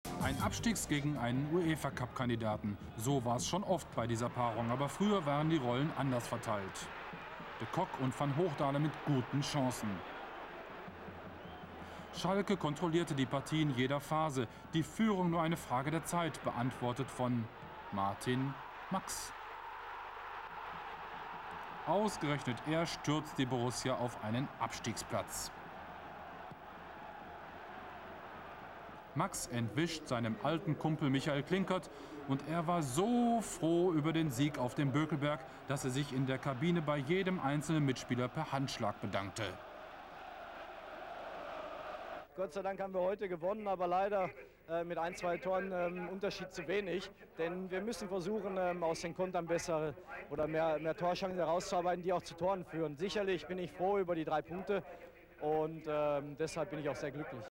22. Spieltag - 1997/1998 - Borussia Mönchengladbach - FC Schalke 04 0:1
22. Spieltag, 7.2.1998
Borussia Mönchengladbach - FC Schalke 04 0:1 (0:1),
Schalke: Lehmann - Thon - De Kock, Linke - Latal (80.Müller), Nemec, Wilmots, van Hoogdalem, Büskens - Max (88.Held), Eijkelkamp (61. Eigenrauch),
Tore: 0:1 Max (42.)